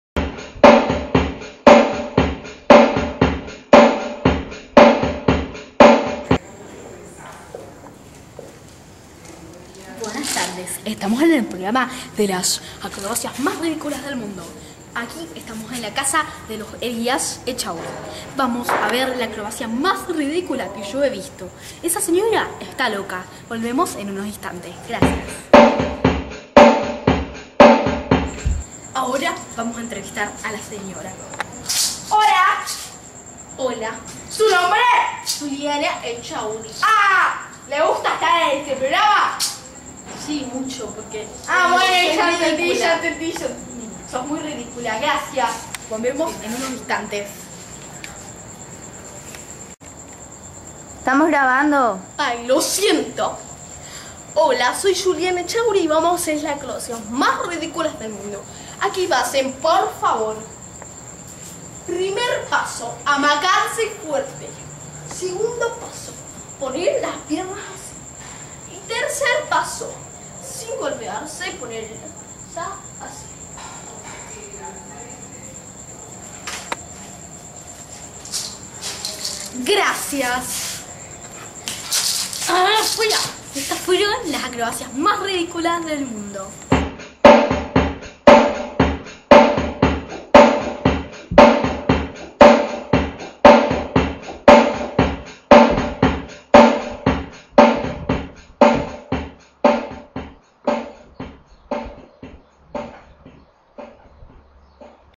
Las Acrobacias Mas ridiculas del Mundo - Selec Studios
Programa cómico que muestra las acrobacias más ridículas del mundo =)